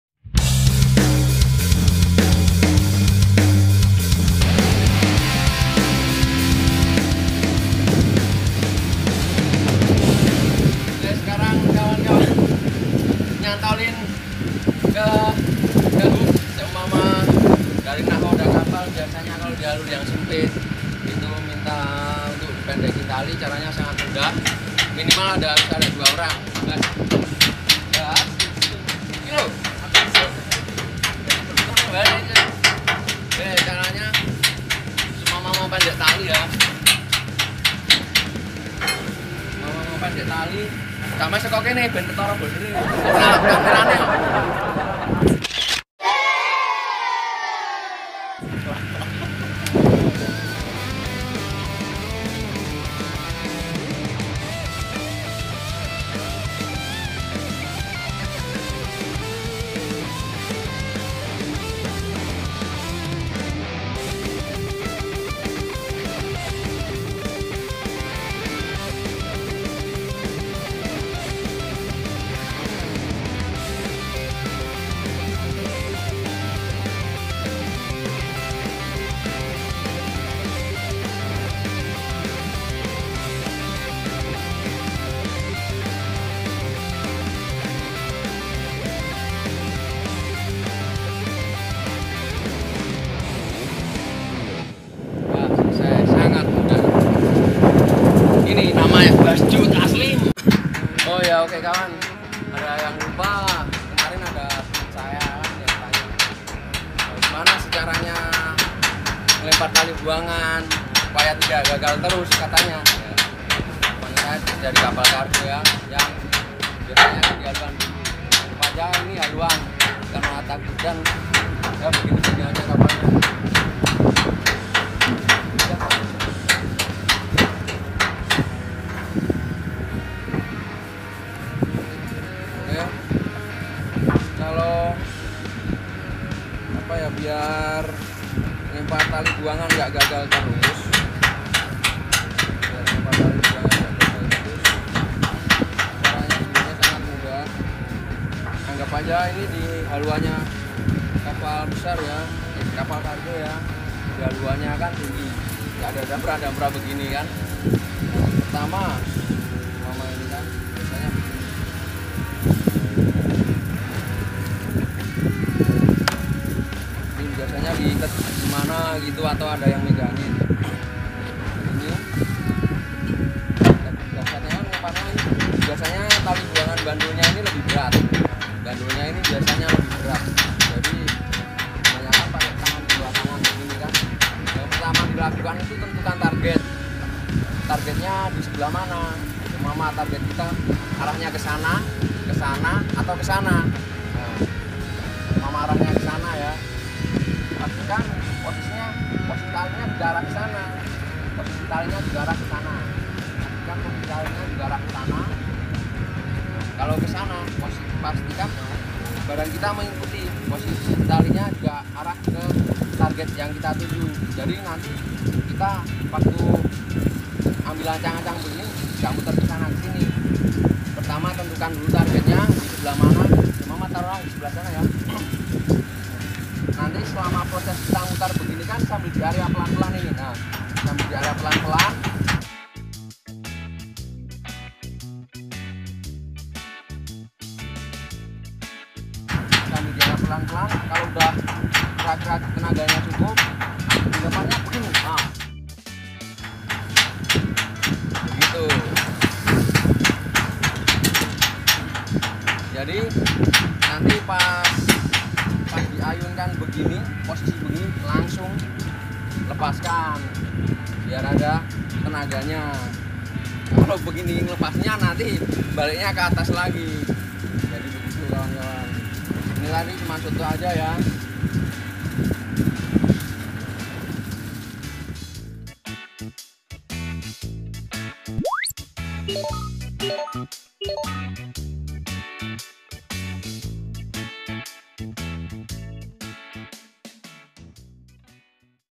Cara mudah, Membelitkan Tali ke Hook | teknik Melempar tali buangan.
Hallo para pelaut indonesia dimanapun berada..
Pada kesempatan kali ini, kami ingin membagikan sebuah video Tutorial :
▪Cara Mudah Membelitkan tali ke Hook
▪Dan teknik melempar Tali Buangan
Semoga video dari kami bisa bermanfaat dan menjadi bahan yang menarik untuk di tonton, 😊
Jangan lupa berdoa dan slalu berhati hati dalam bekerja.
"Salam pelaut indonesia"

Terimakasih.